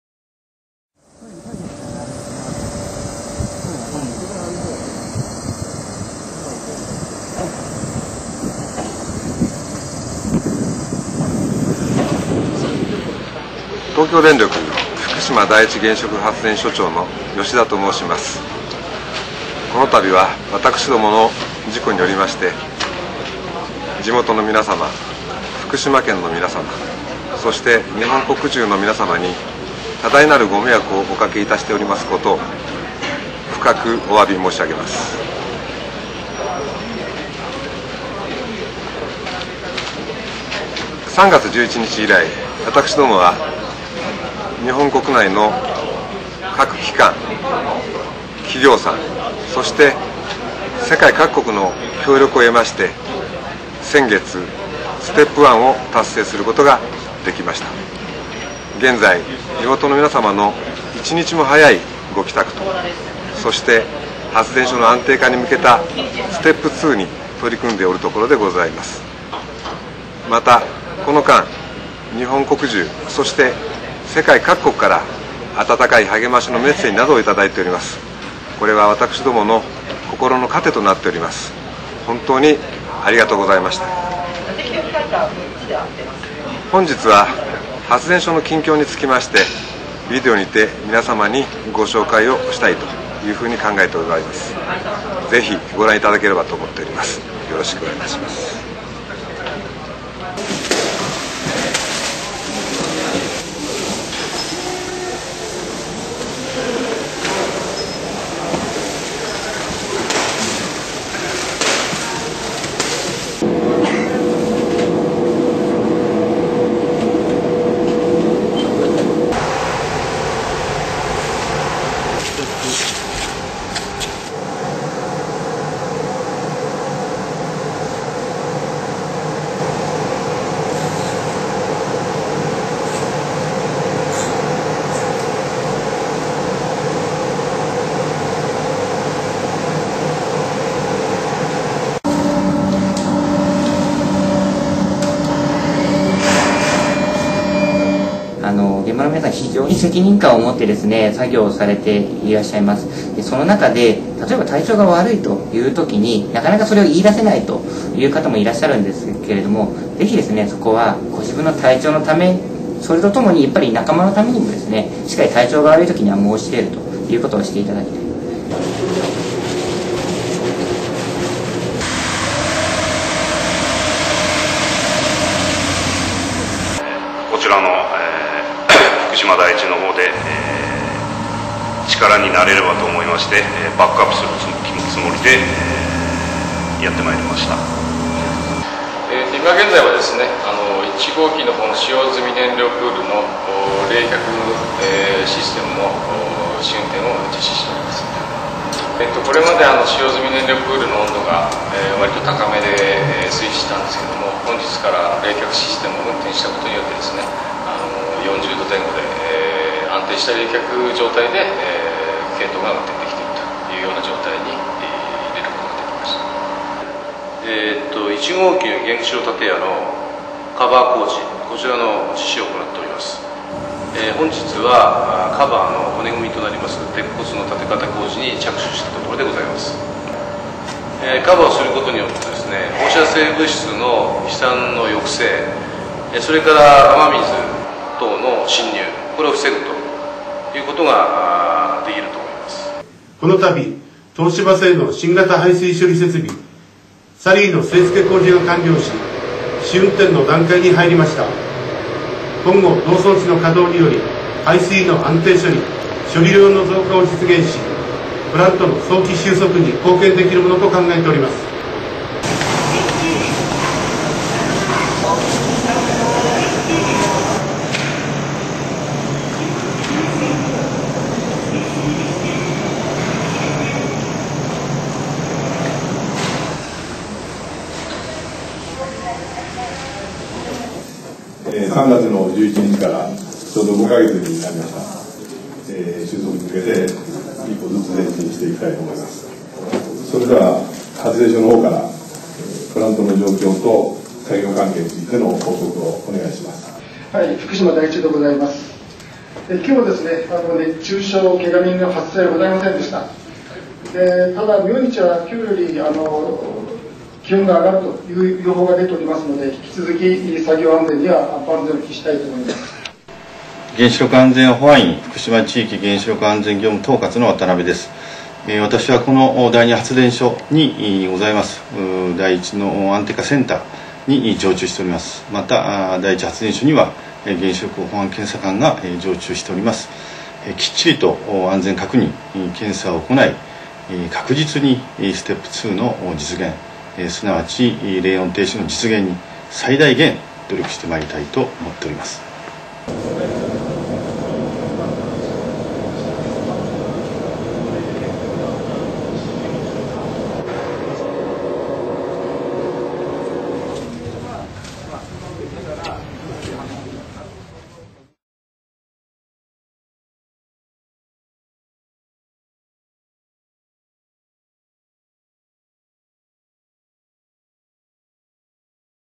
Reporting from the Grounds of Fukushima Daiichi Nuclear Power Station
A TEPCO video release today called Reporting from the Grounds of Fukushima Daiichi Nuclear Power Station